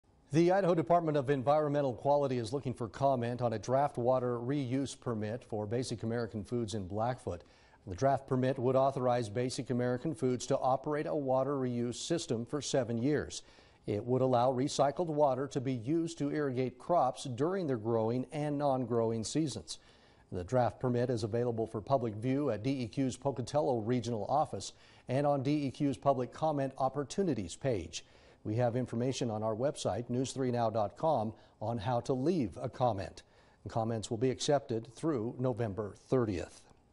DEQ seeks comment on draft reuse permit for Basic American Foods in Blackfoot
DEQ seeks comment on draft reuse permit for Basic American Foods in Blackfoot [(954762)]